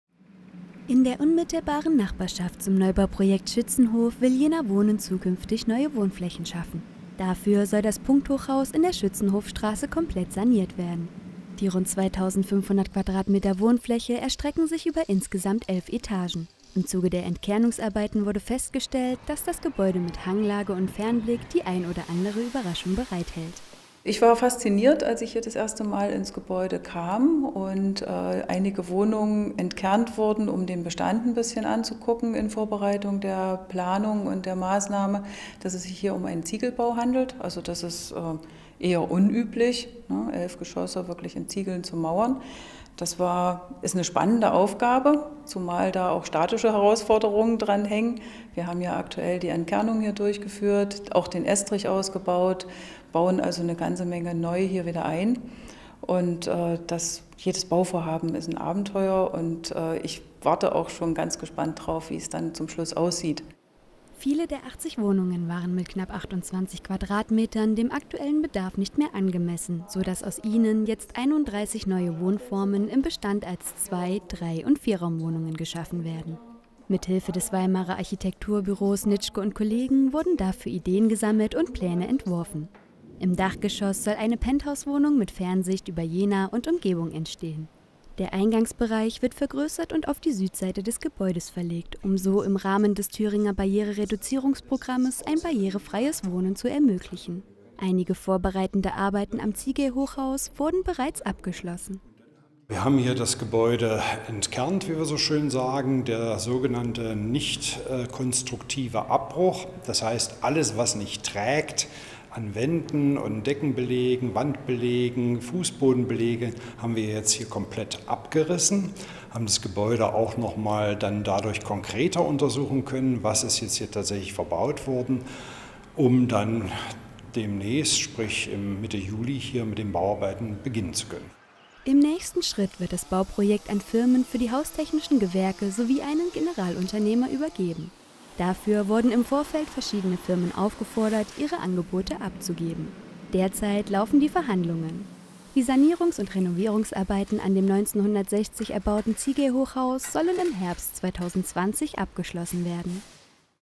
Sanierung Schützenhofstrasse (Teil 2/2019)
Aus alt mach neu: In der Schützenhofstraße wird ein Hochhaus entkernt, Pläne für neue und größere Wohnungen gemacht sowie neuer Wohnraum geschaffen.